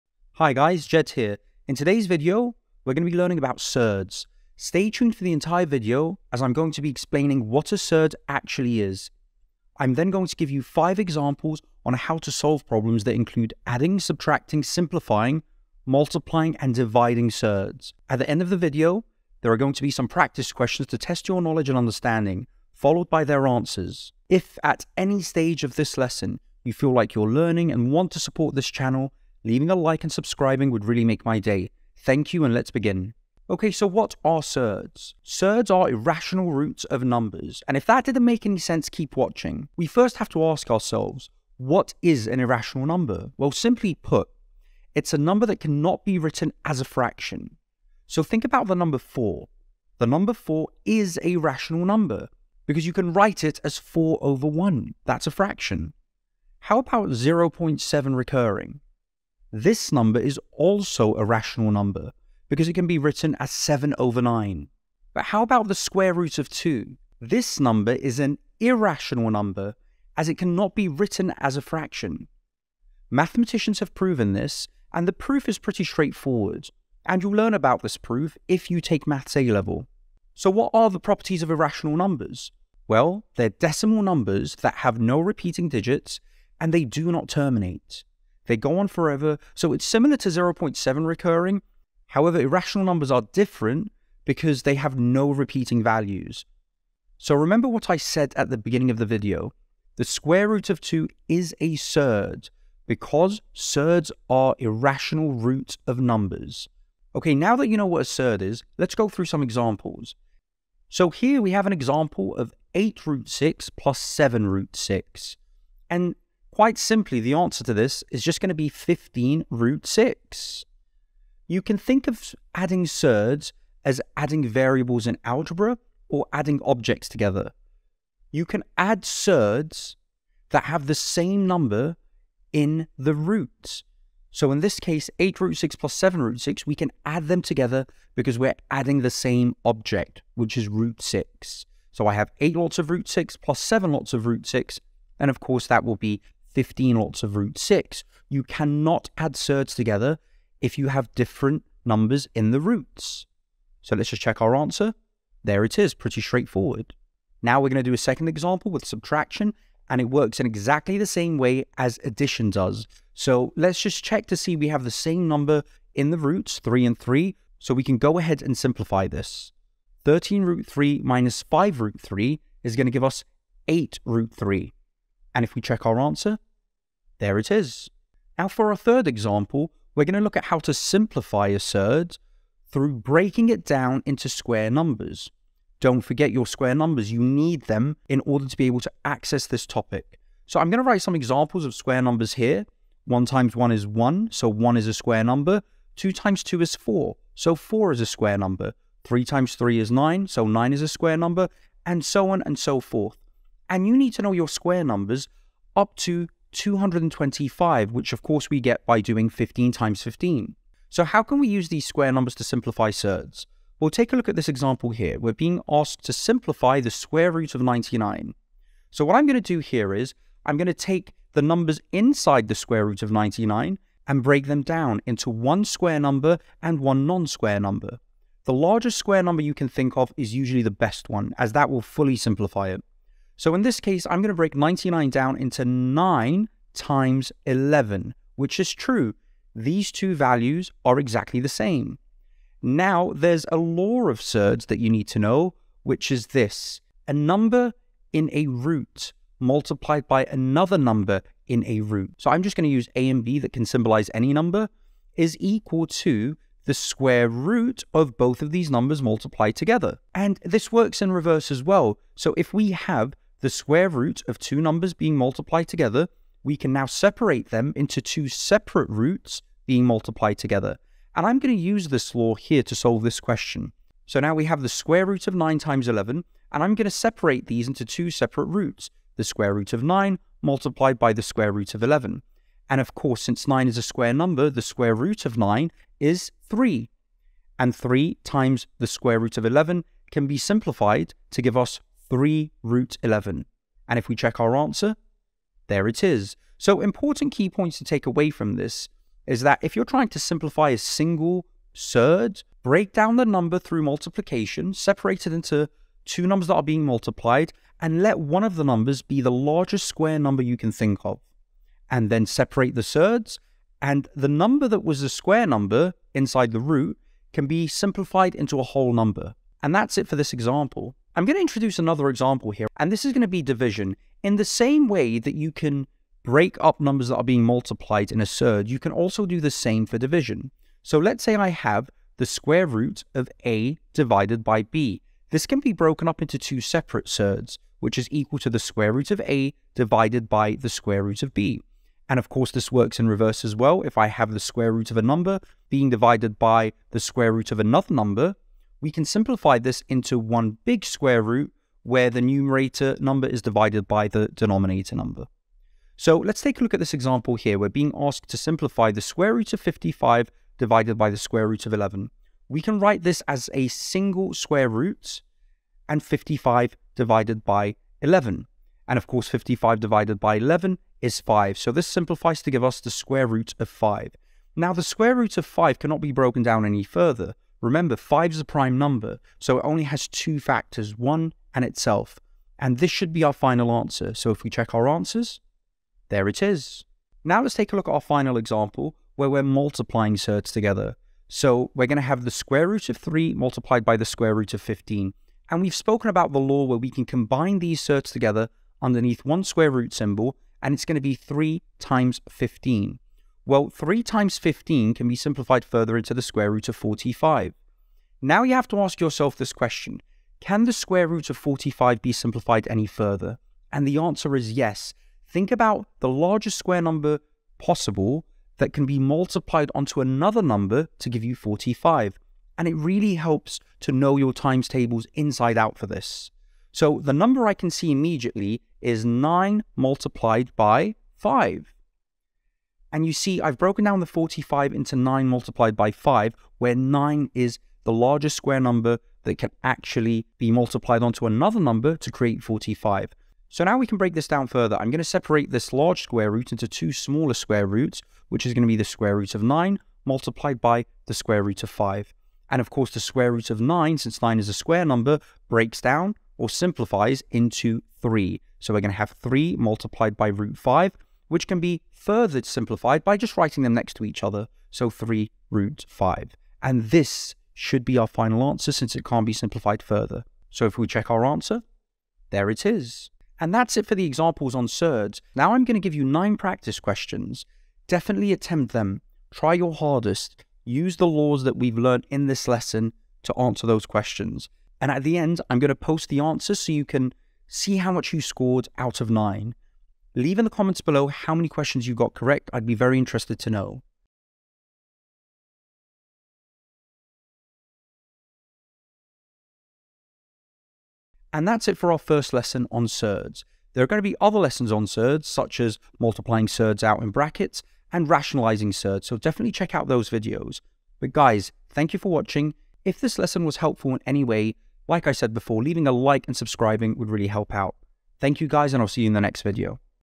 Surds Introduction - GCSE Maths
In this lesson, I will teach you what a surd is, how to add, subtract, simplify, multiply and divide them.

We hope you gain knowledge and a deeper understanding of this topic by watching this video.